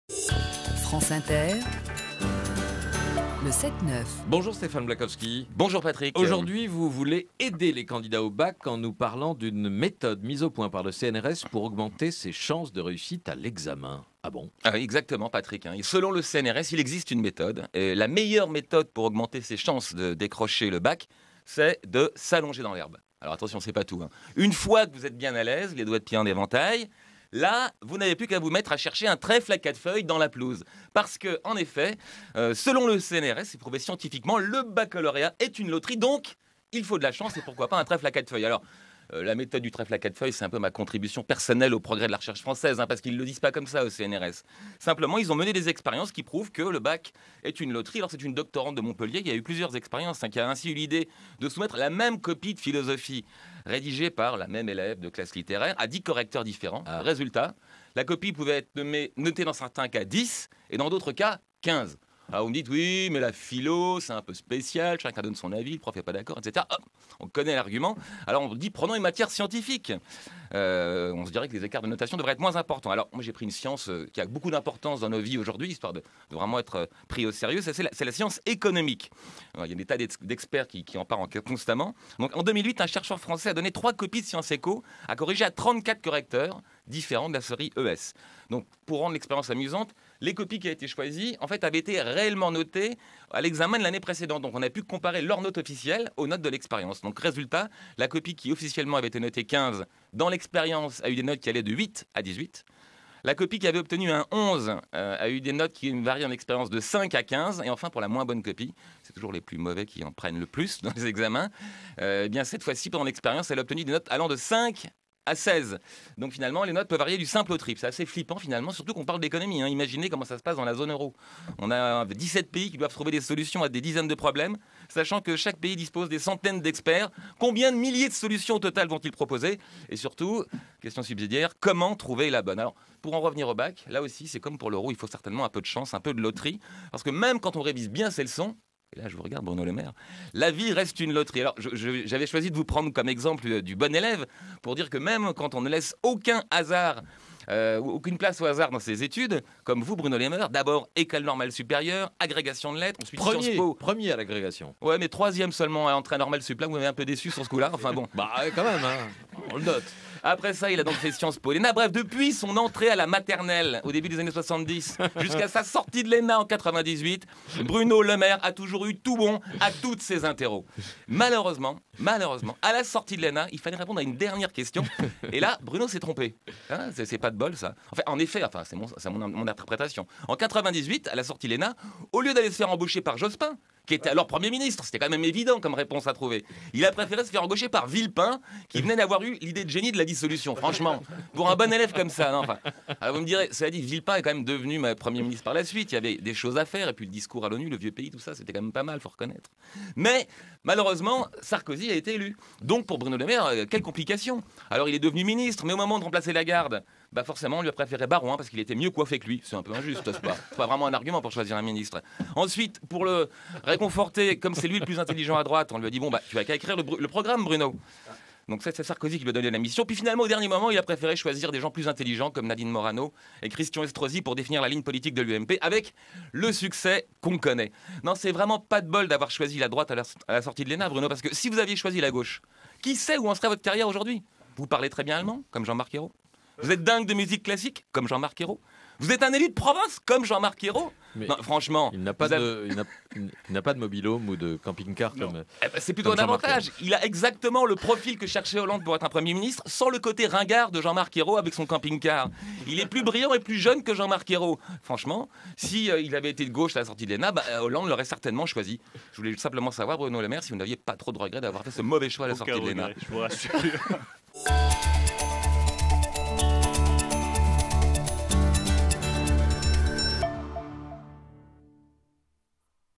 Le Bac est une loterie - La chronique de Stéphane Blakowski
En ces temps stressants pour les jeunes étudiants, Stéphane Blakowski veut aider les candidats au bac face à la loterie qu'est le fameux diplôme. Le bac est une loterie, c'est pas lui qui le dit mais le CNRS (8h55 - 19 juin 2012) !!